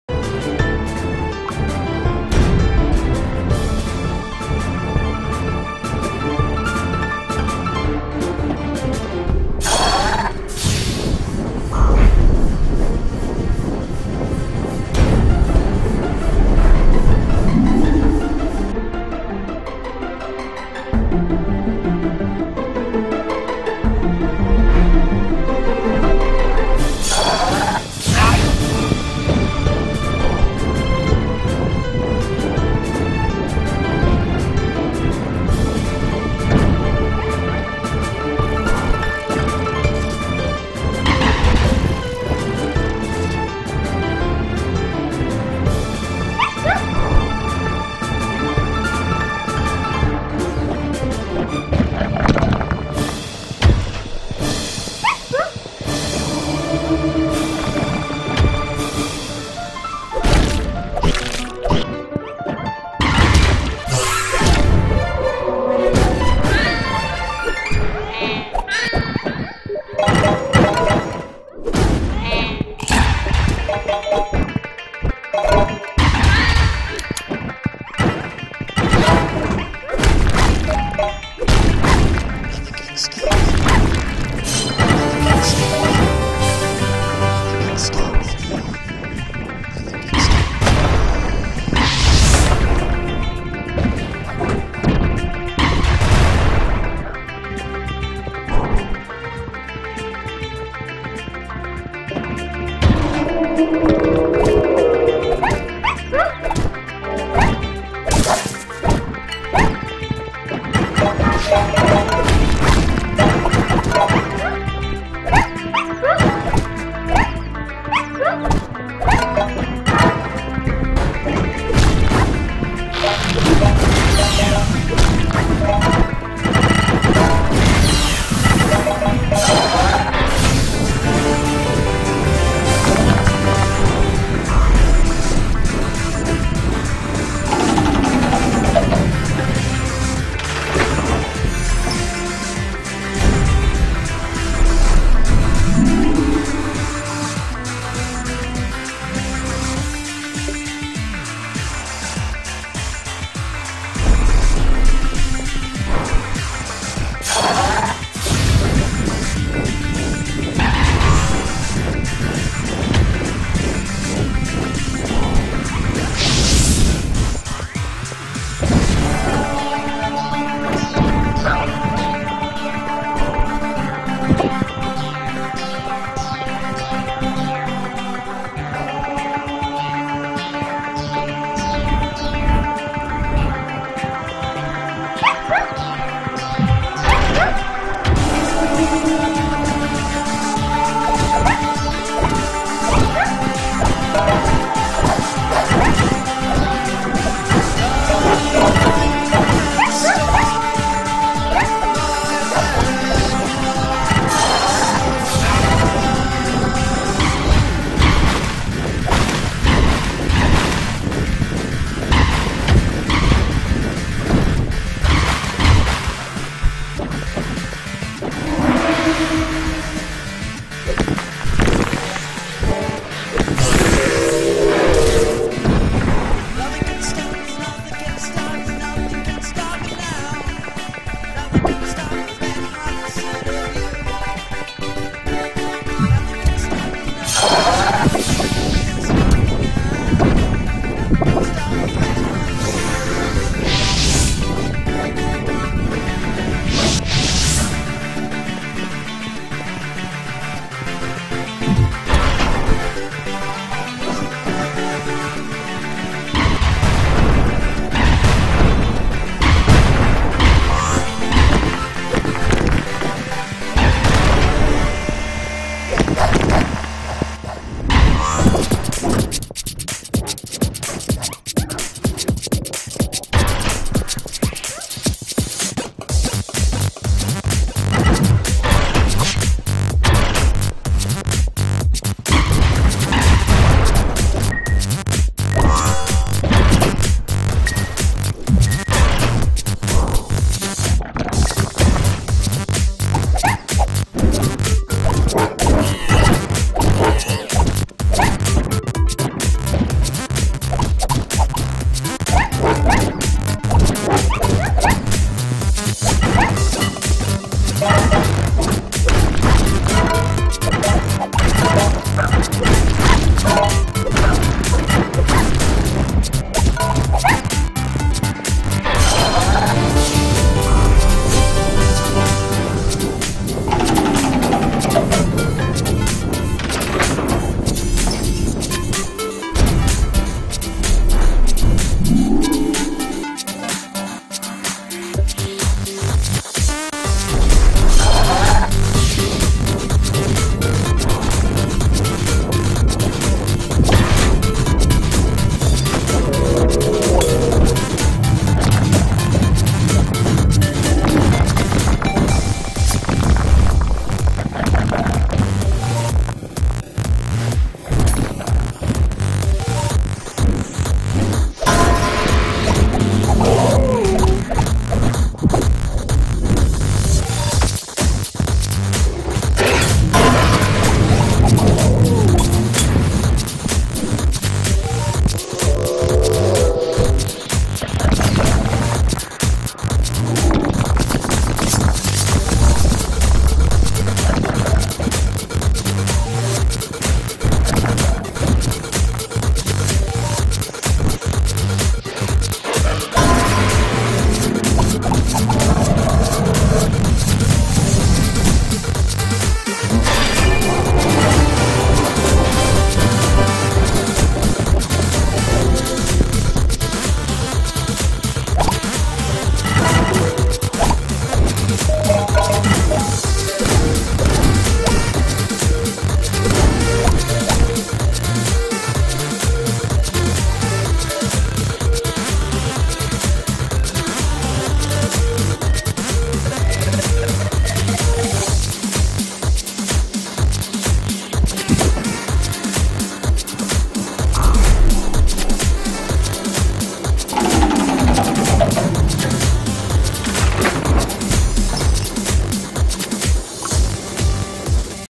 [PvE] Gloaming Wildwoods in 6:15
After a few runs I managed to get this time, which could definitely be cut down by a half a minute or so but I don't think I'll have any interest in that given how this run isn't challenging in any way at all.